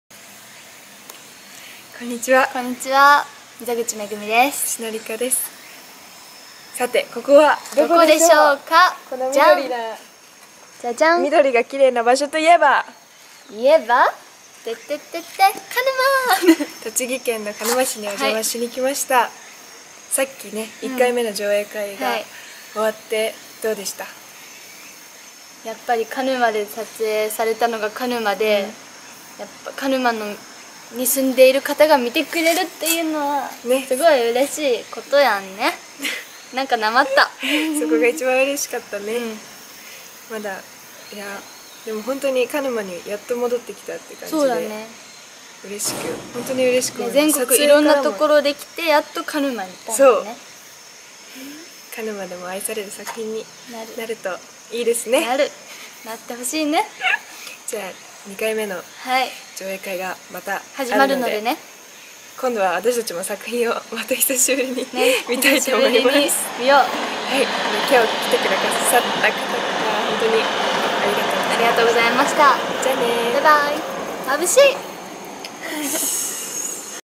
栃木県鹿沼市にて（前編）
映画「ネコヤドのハルとアキ」「ソラから来た転校生」の上映会が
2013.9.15に映画のロケ地にもなった栃木県鹿沼市でありました。